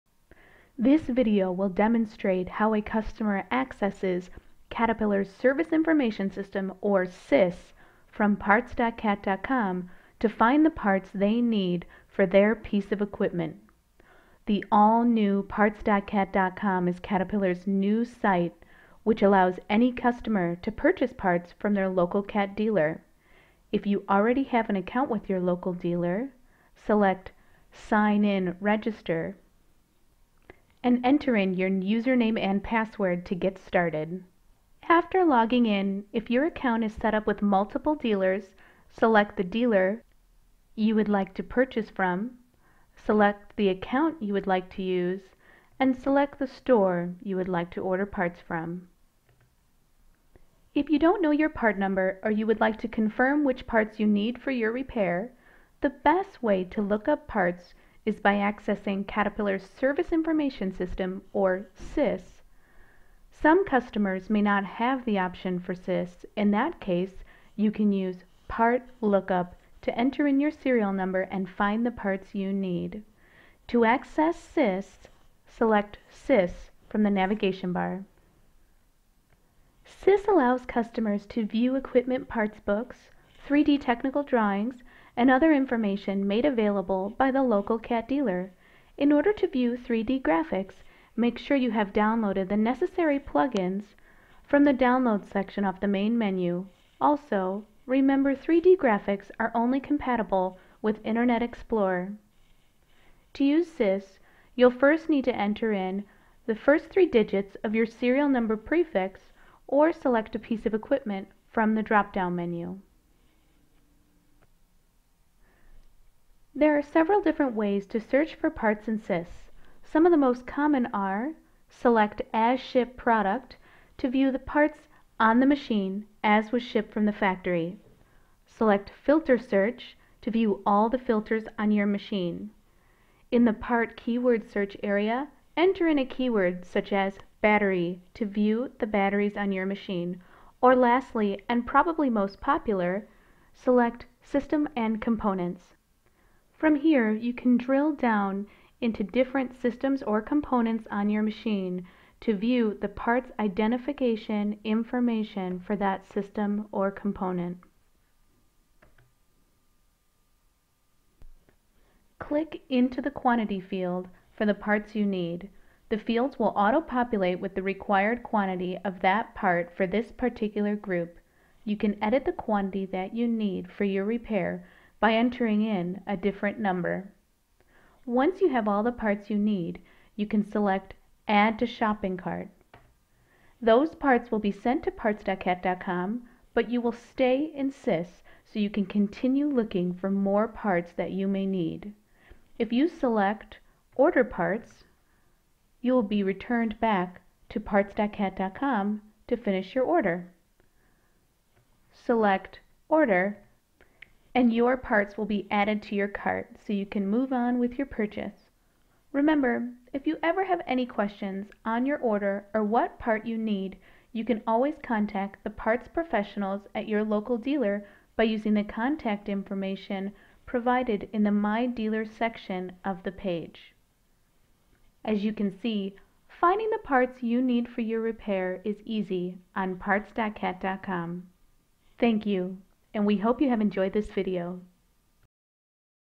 How to find your parts in SIS on Parts.Cat.Com
You've got a job to do and it doesn't involve wasting time. That's why we developed Parts.Cat.Com, the next generation of PartStore™. Designed to be the fastest, easiest, and most reliable place to find genuine Cat parts online.